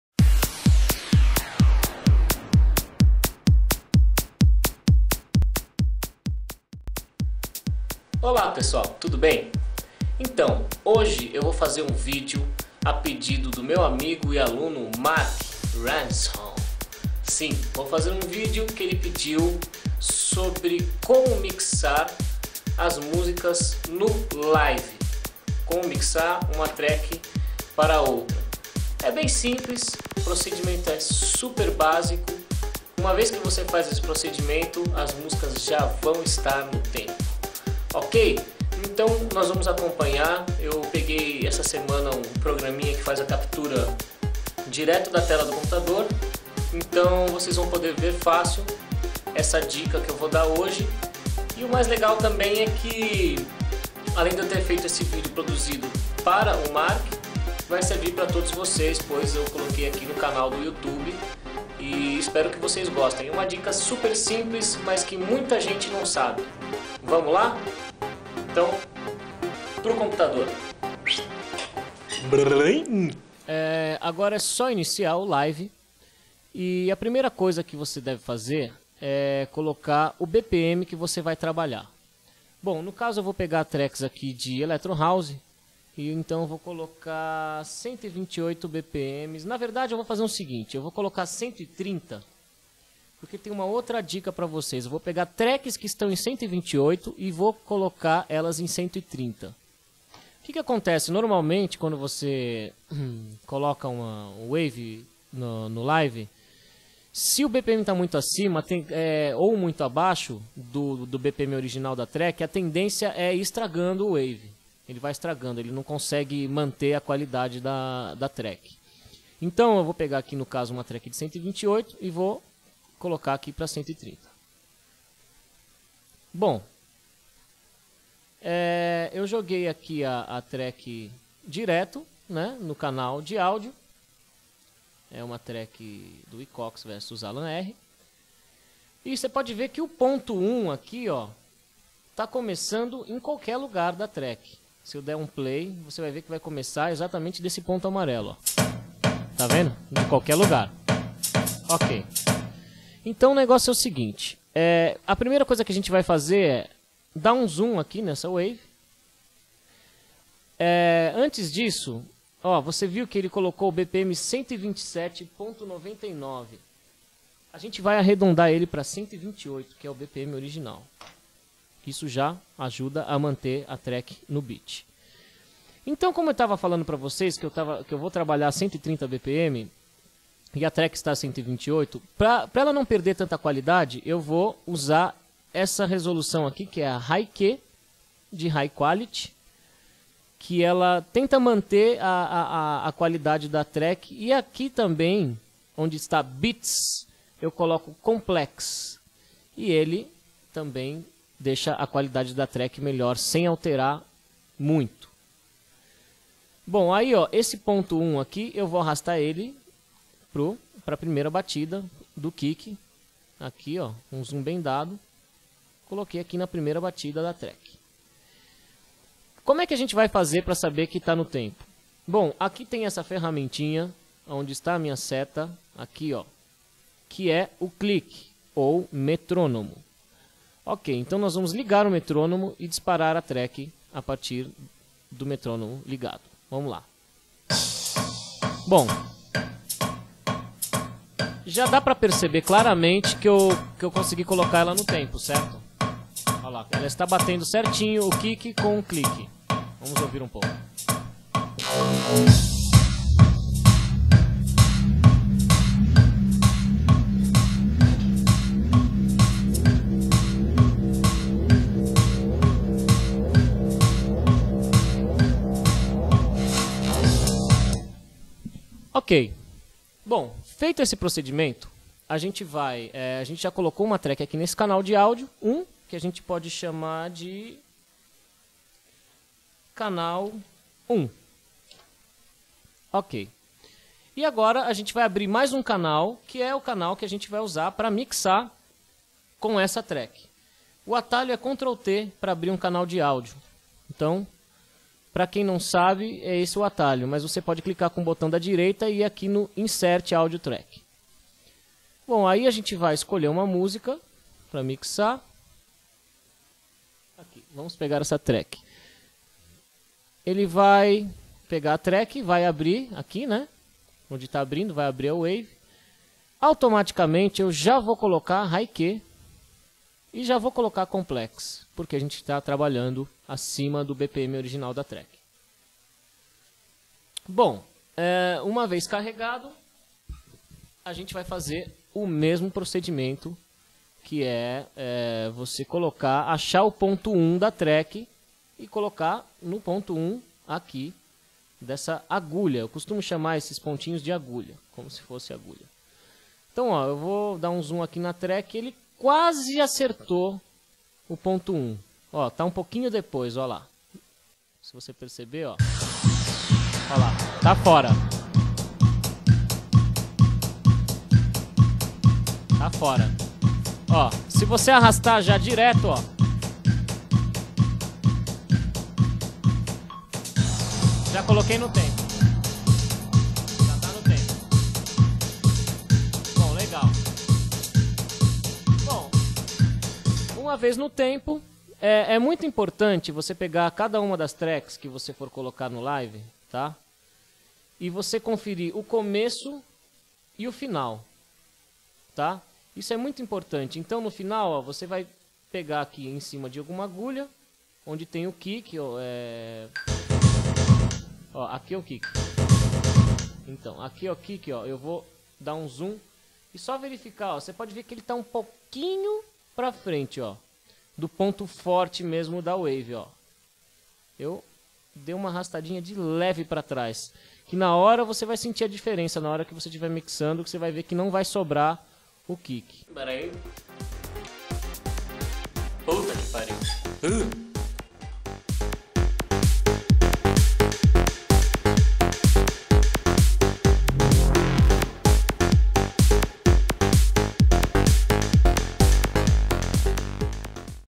Dj Tutorial - Mixando Tracks no Ableton Live, parte 1
Ola pessoal, nesses videos eu mostro um pouco de como se mixar as tracks no Ableton Live. É  uma coisa bem simples e facil. Espero que voces gostem! Abraços,
Igor Willcox